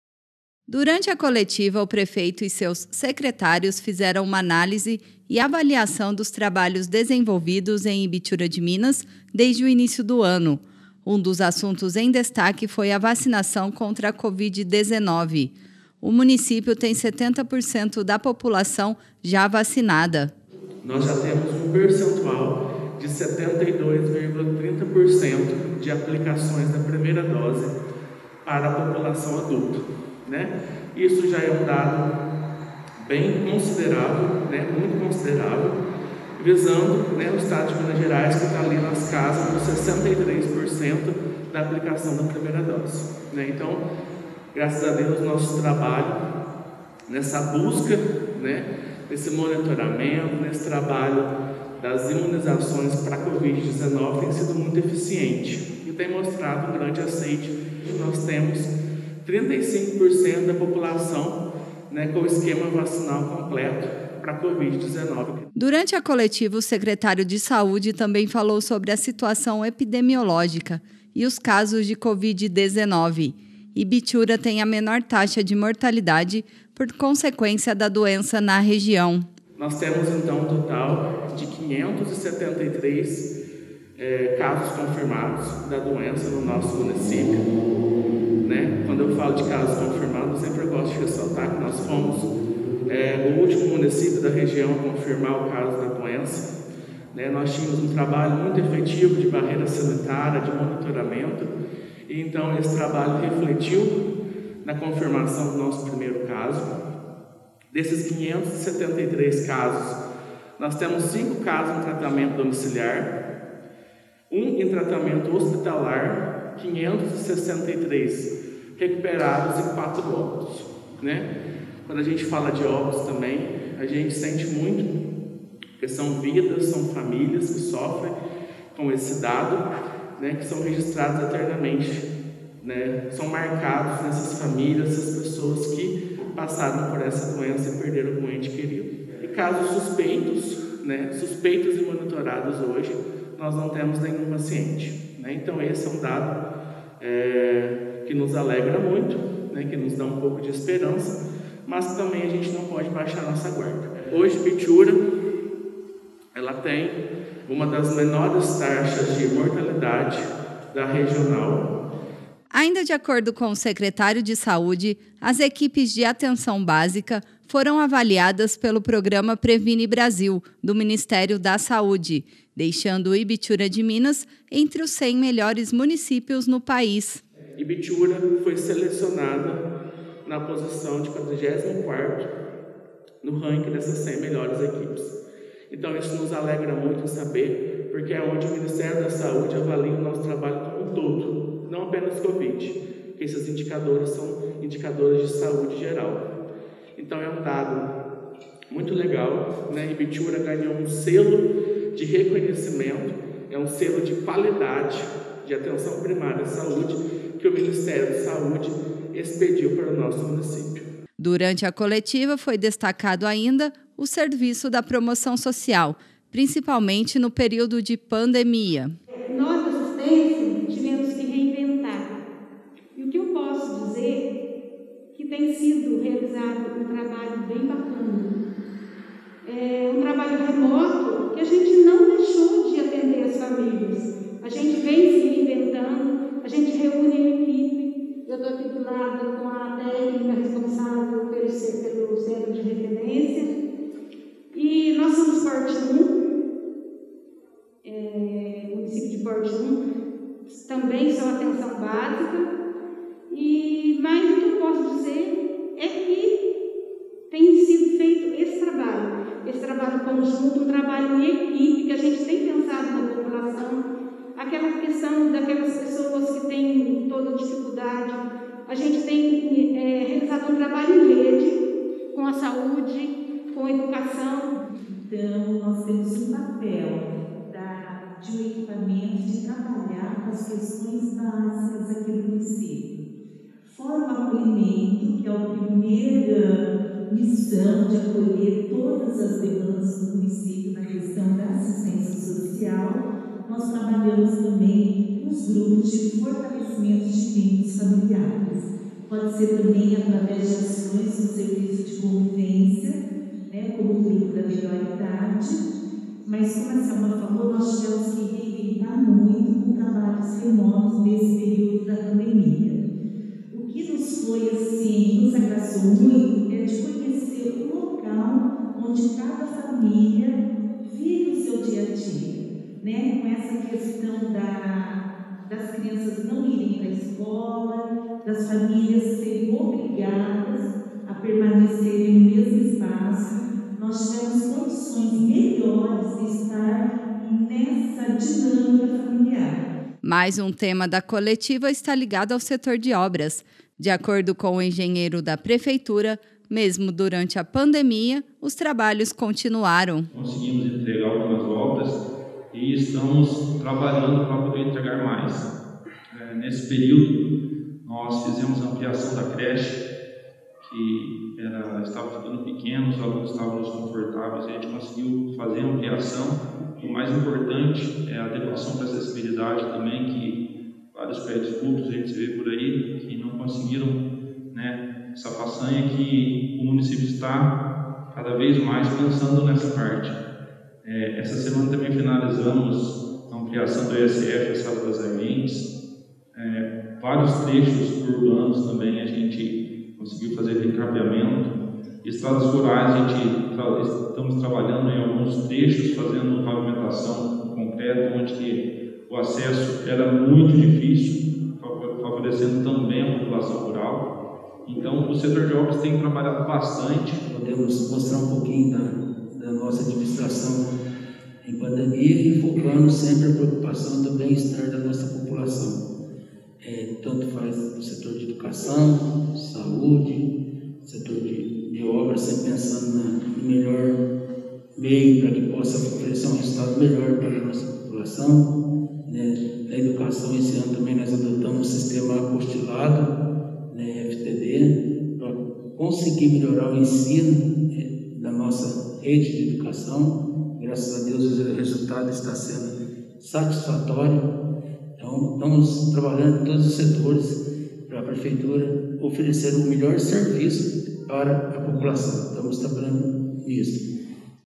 Ibitiura de Minas: Covid 19 e outros assuntos são temas de coletiva
Na última semana o prefeito de Ibitiura de Minas Alexandre Borges convocou a imprensa para uma entrevista coletiva sobre diversos assuntos. Um dos destaques foi a diminuição dos casos de Covid-19 e a menor taxa de mortalidade da região